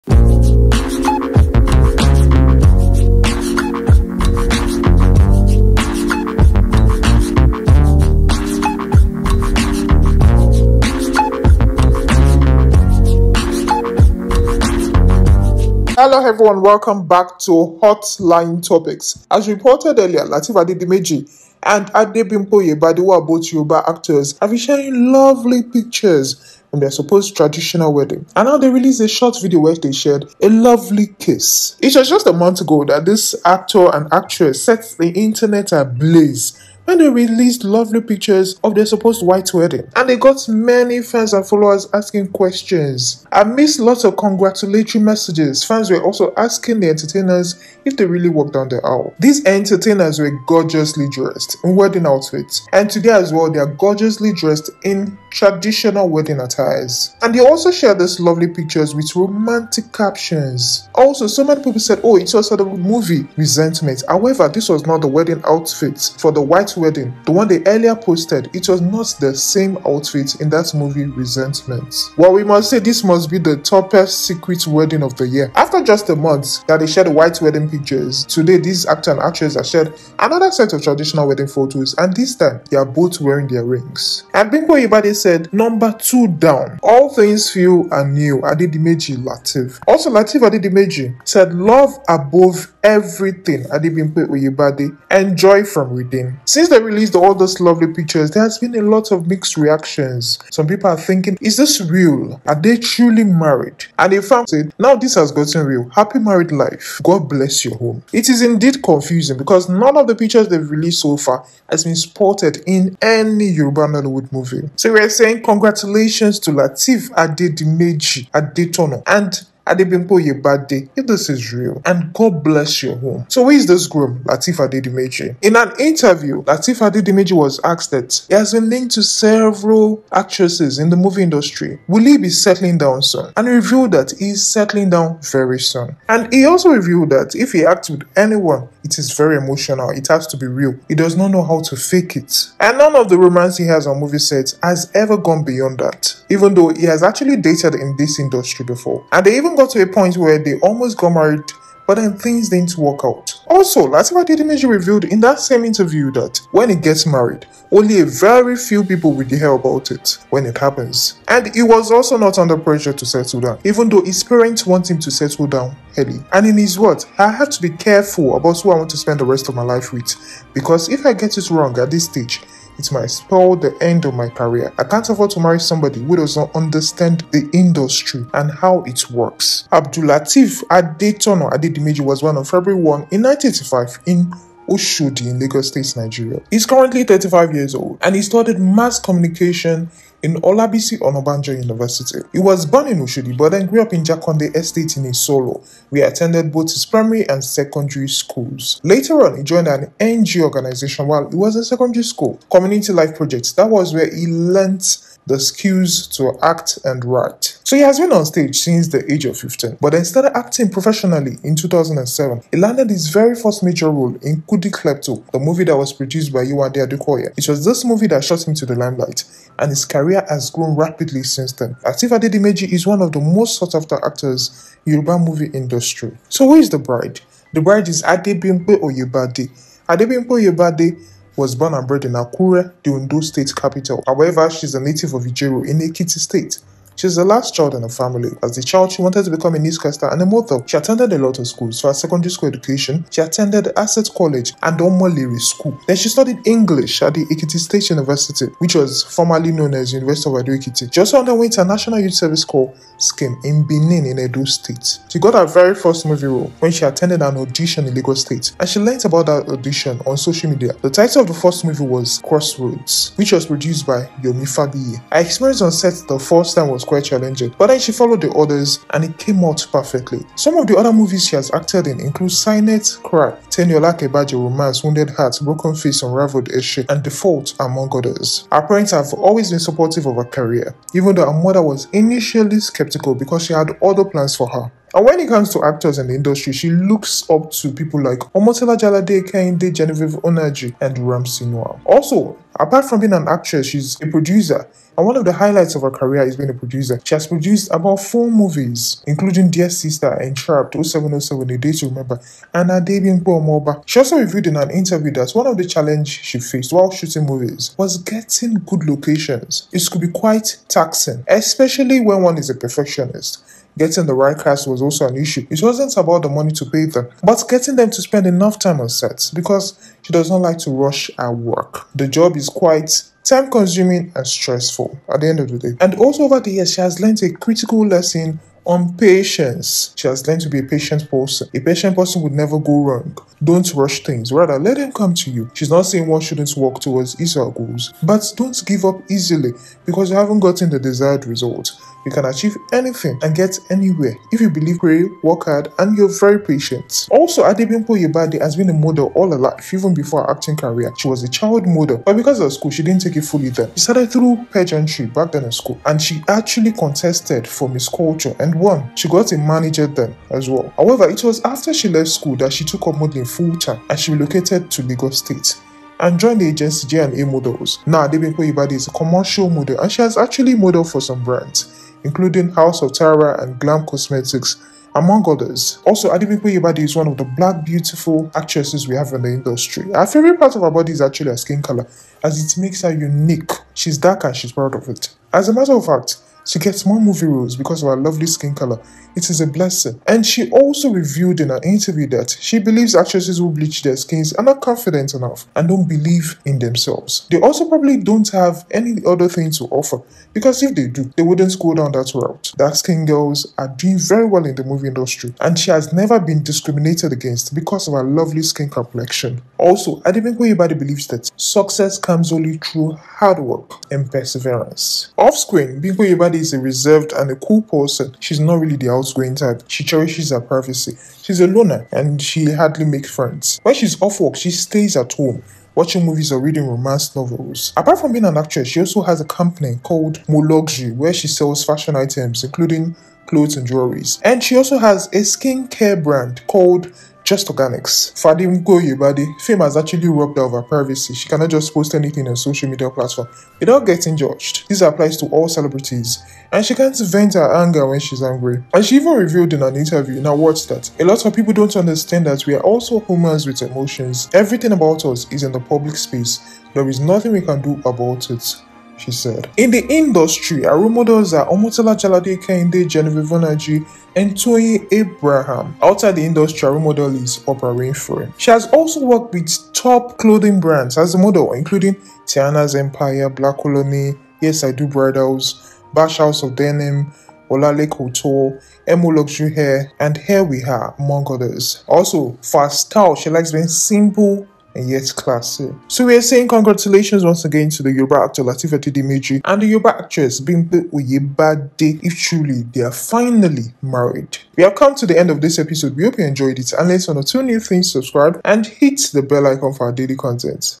Lateef Adedimeji & Bimpe Oyebade Share A Lovely Kiss At Their Secret Traditional Wedding, CONGRATS
Yoruba Actor, Lateef Adedimeji And Yoruba Actress Adebimpe Oyebade Share A Lovely Kiss At Their Secret Traditional Wedding - CONGRATULATIONS & HAPPY MARRIED LIFE